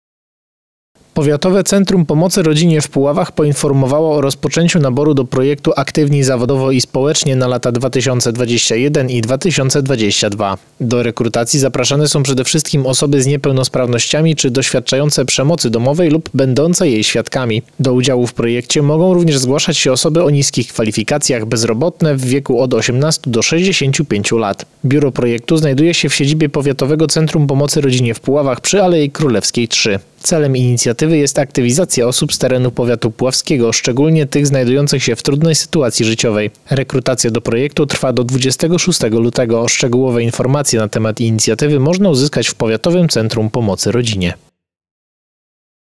15 01 2021 Nowa edycja projektu „Aktywni zawodowo i społecznie”
Celem inicjatywy jest aktywizacja osób z terenu powiatu puławskiego, szczególnie tych znajdujących się w trudnej sytuacji życiowej.